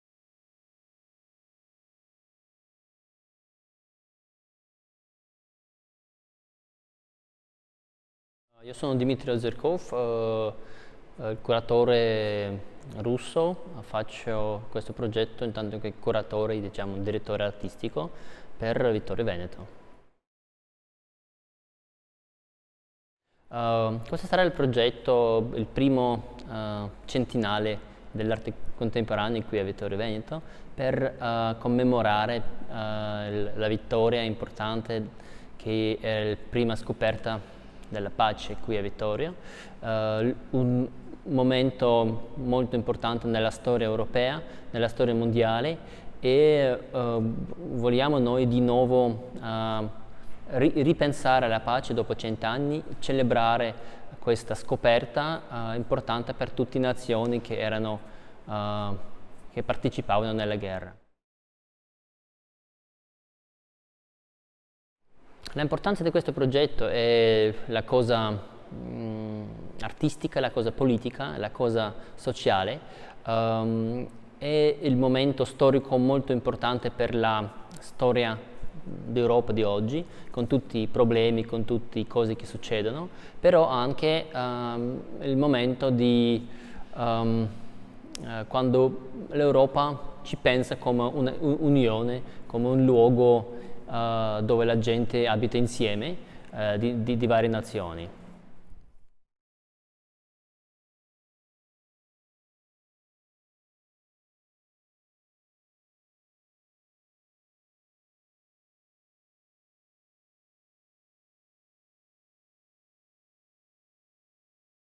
Young International Artists - Intervista al curatore Dimitri Ozerkov
In occasione del Centenario dalla fine della Prima Guerra Mondiale, 13 giovani artisti provenienti da 12 nazioni, hanno realizzato 13 diversi progetti per la Città destinati a rimanere patrimonio di Vittorio Veneto. Gli storytellers del Progetto Giovani di Vittorio Veneto hanno seguito e raccontato questo progetto collettivo che si fa arte pubblica.